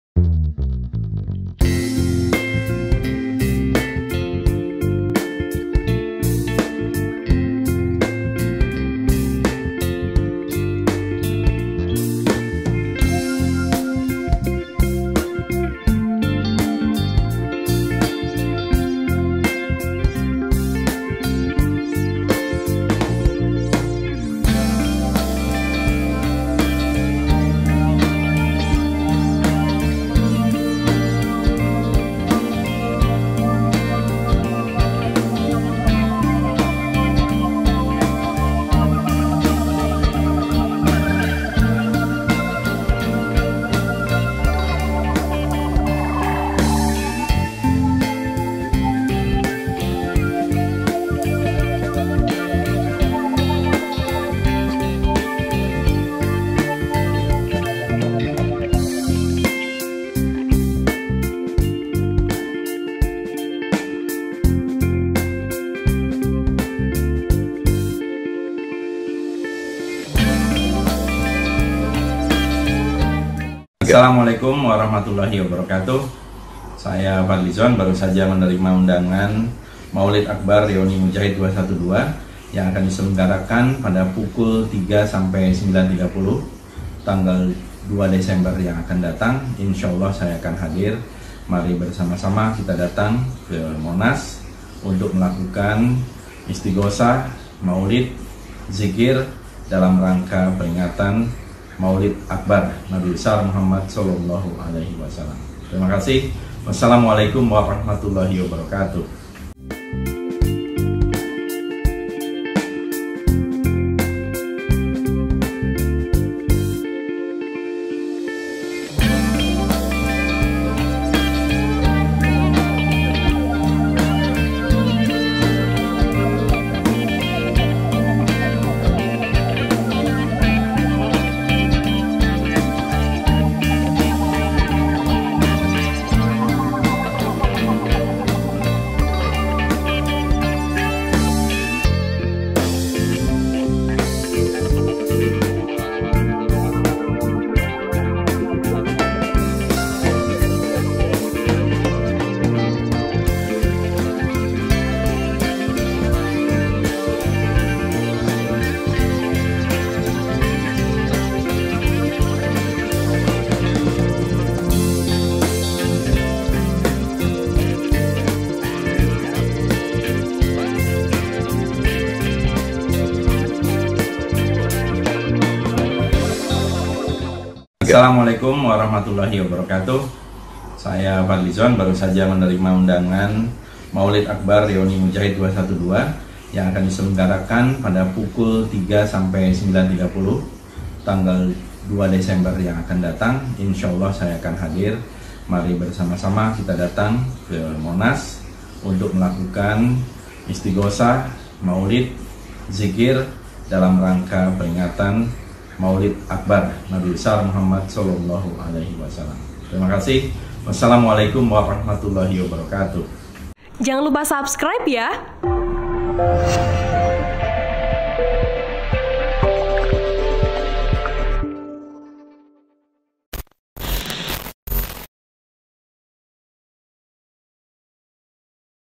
Fadli Zon Pamer Undangan VIP Reuni Akbar 212 di Twitter, Nyatakan Akan Hadir
TRIBUN-VIDEO.COM - Wakil Ketua Umum Gerinda, Fadli Zon mengatakan akan menghadiri acara reuni akbar 212 pada, Senin (2/12/2019) di Monas, Jakarta.

Pernyataan tersebut diunggah di akan twitternya @fadlizon pada Sabtu (30/11/2019).

Dalam video yang diunggah, Fadli Zon menunjukkan undangan VIP untuk acara reuni 212.